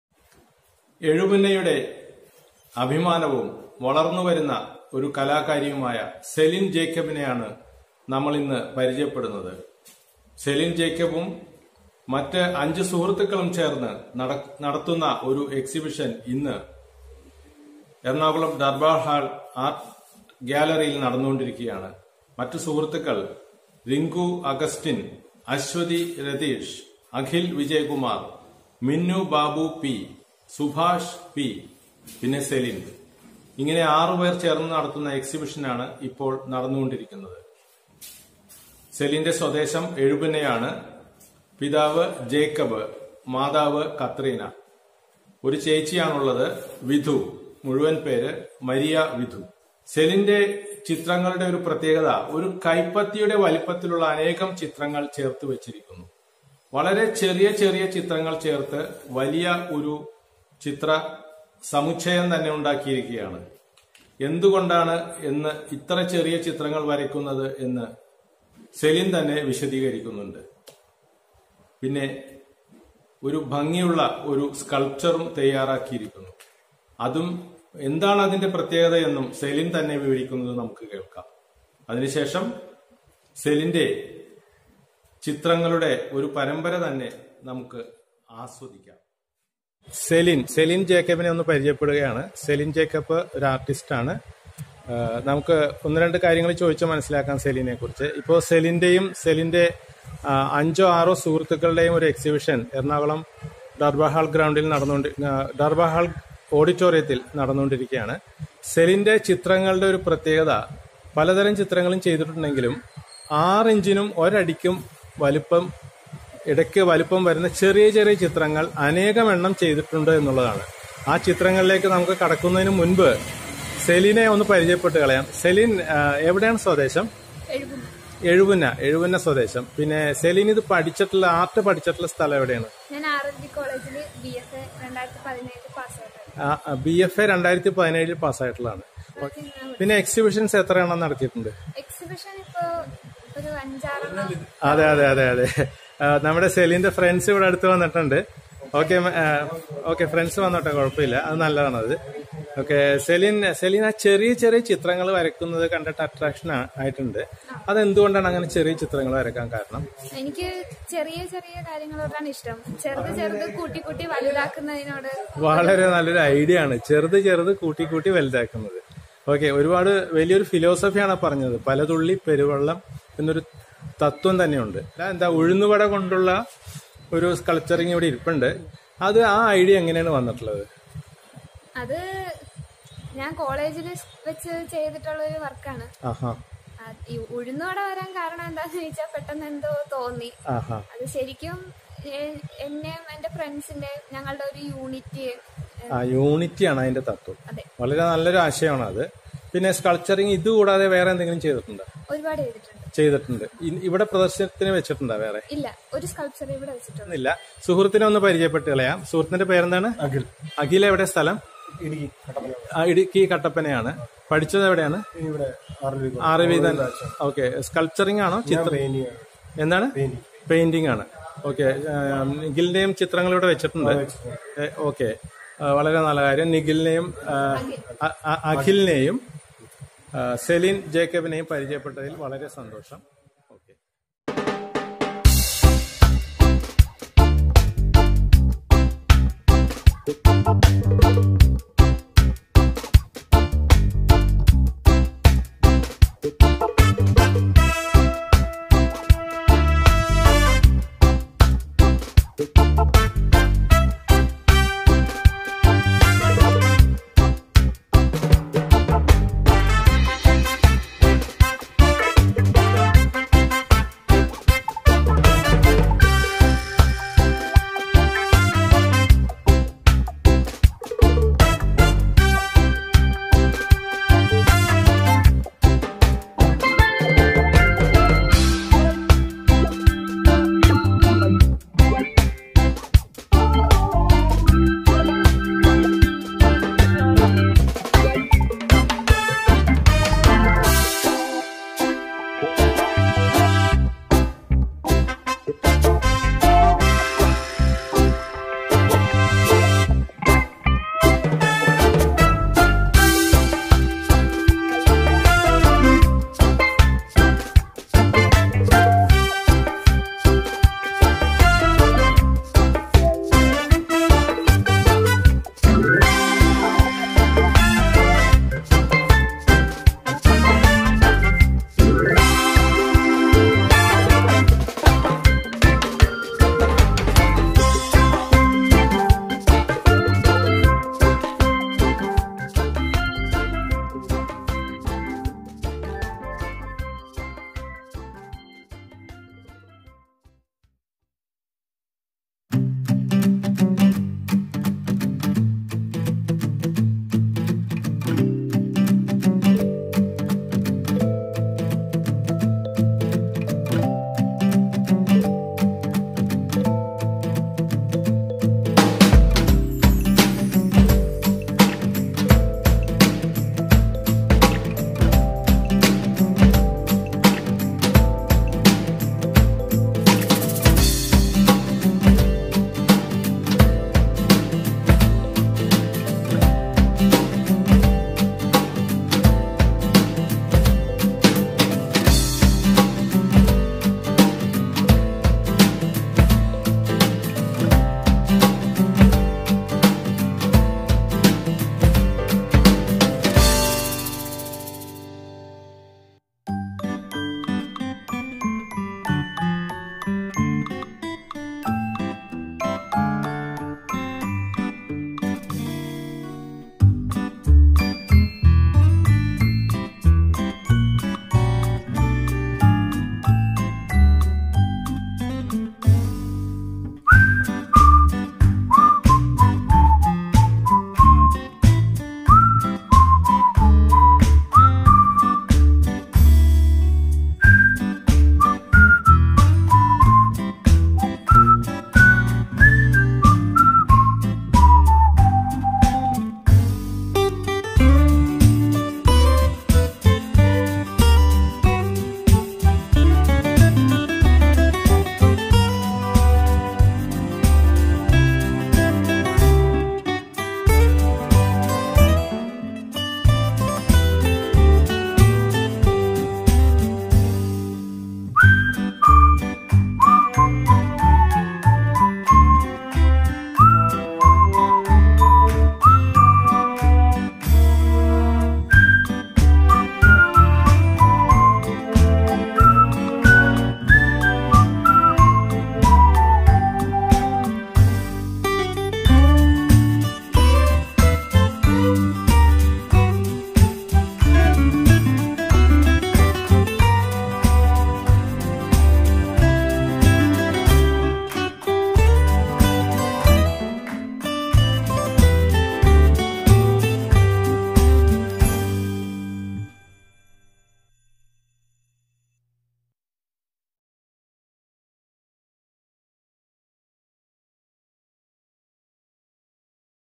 Celin Jacob V. gifted artist from EZHUPUNNA
Celine Jacib and her five artist friends: Rinku Augustine, Aswathy Rathish, Akhil Vijayakumar, Minnu Babu.P and Subhash.P jointly conducts Paintings and Sculptures Exhibition from 01 to 06 September 2018 at Durbar Hall Art Gallery.